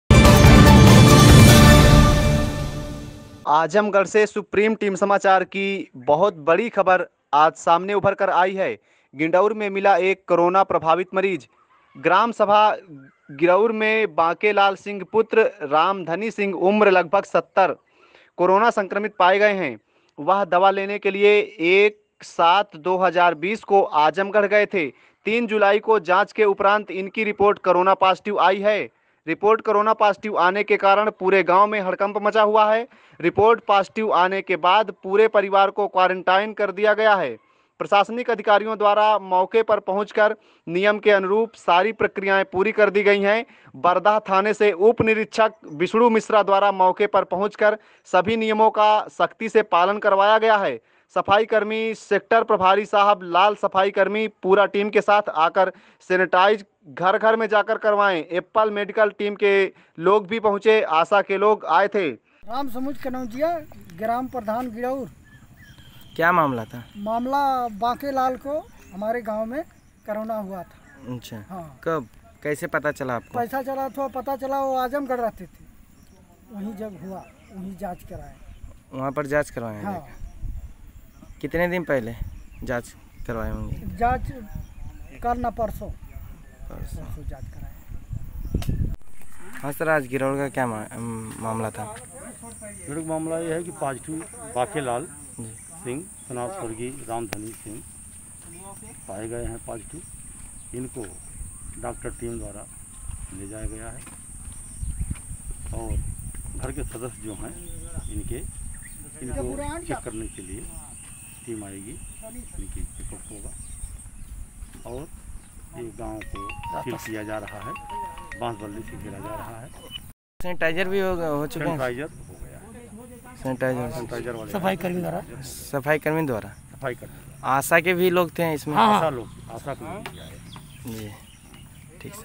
आजमगढ़ में ग्राम गिड़उर क्षेत्र में एक व्यक्ति कोरोना पॉजिटिव मिला l Supreme Samachar l
ग्राम सभा गिरउर मे बांकेलाल सिंह पुत्र रामधनी सिंह उम्र लगभग 70 कोरोना संक्रमित पाए गए हैं वह दवा लेने के लिए 1-7-20
  को आजमगढ़ गए थे ,3 जुलाई को जांच के उपरांत इनकी रिपोर्ट करोना पॉजिटिव आई है , रिपोर्ट कोरोना पॉजिटिव आने के कारण पूरे गांव में हड़कंप मचा हुआ है
       रिपोर्ट पॉजिटिव आने के बाद पूरे परिवार को क्वॉरेंटाइन कर दिया गया है प्रशासनिक अधिकारियों द्वारा मौके पर पहुंचकर नियम के अनुरूप सारी प्रक्रियाएं पूरी कर दी गई है  बरदह थाने से उप निरीक्षक विष्णु मिश्रा द्वारा मौके पर पहुंचकर सभी नियमों का सख्ती से पालन करवाया गया है सफाई कर्मी सेक्टर प्रभारी साहब लाल सफाई कर्मी  पूरा टीम के साथ आकर सेनीटाइज घर घर में जाकर करवाएं एप्पल मेडिकल टीम के लोग भी  पहुंचे  आशा के लोग  आए थे